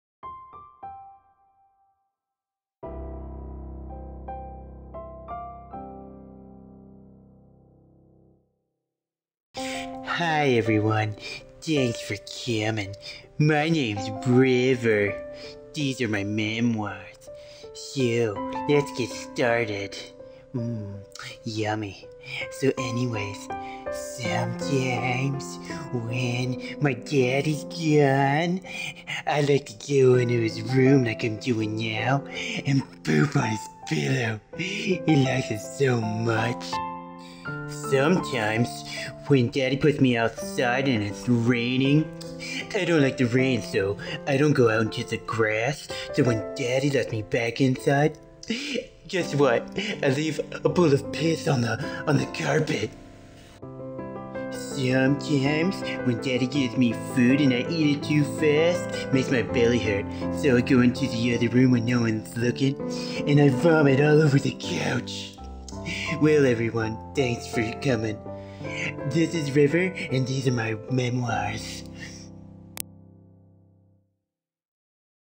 Funny talking dog video - rivers memoirs
My Brothers dog she is a shiba inu..we made a silly voice for her one night when we were having a house party.bee have been talking about doing a talking dog video for her for a long time. Here it is!